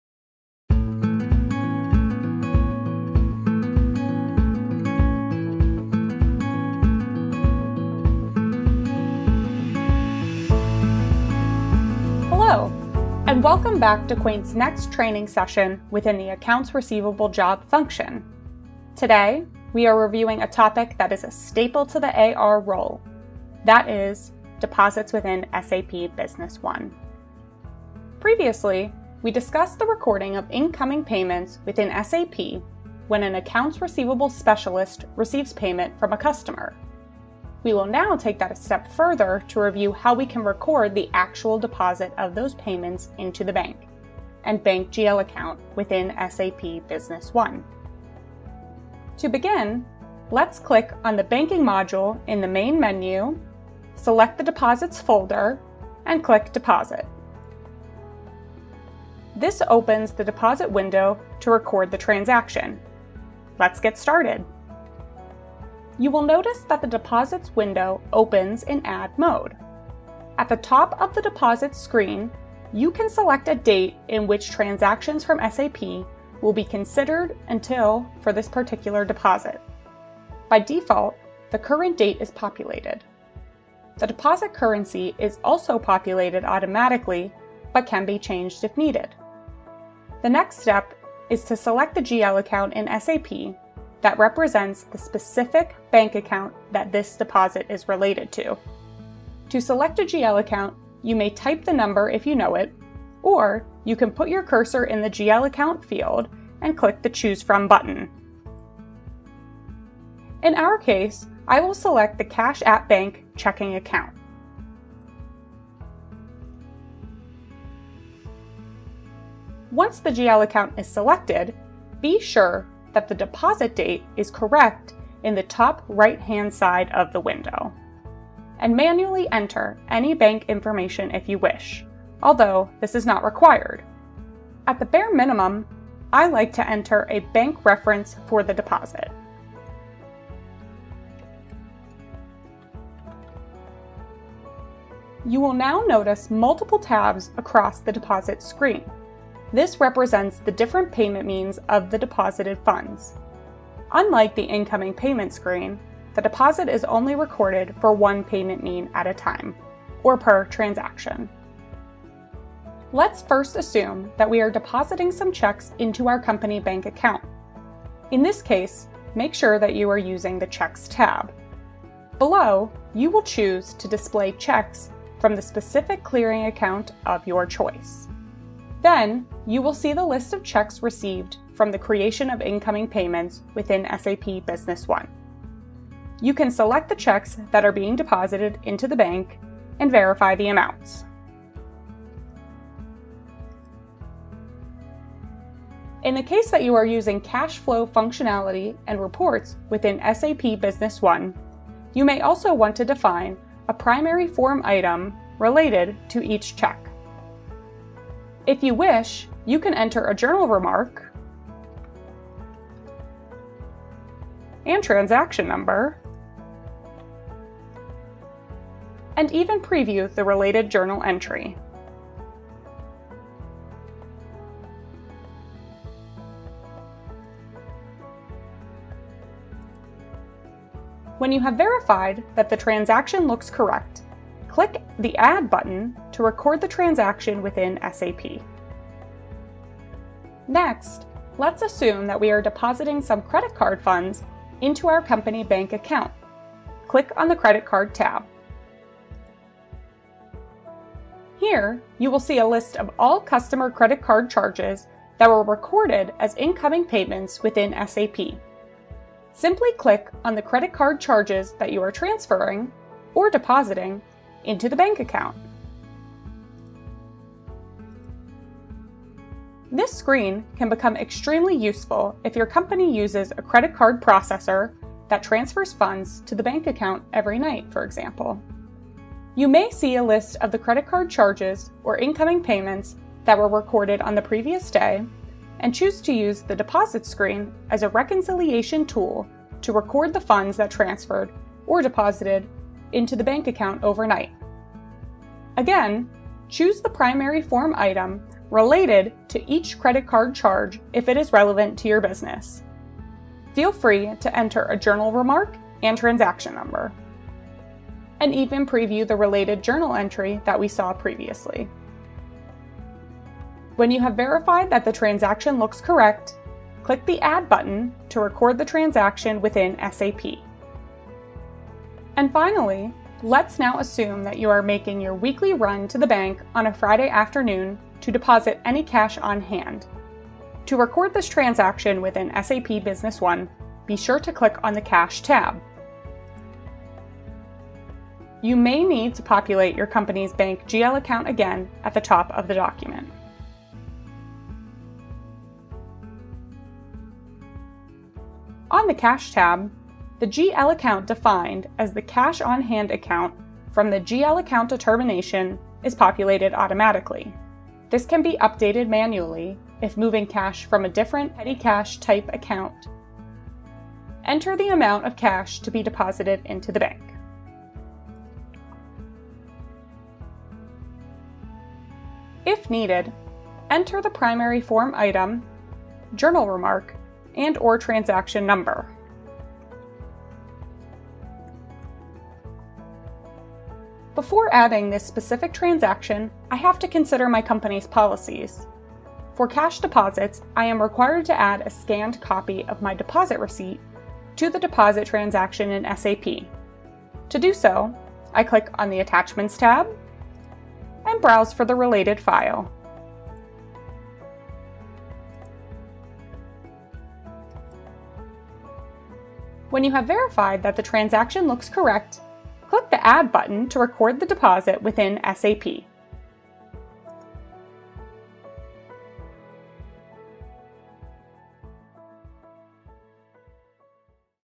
Accounts Receivable - Deposits | SAP Business One
An overview on how to use Deposits within SAP Business One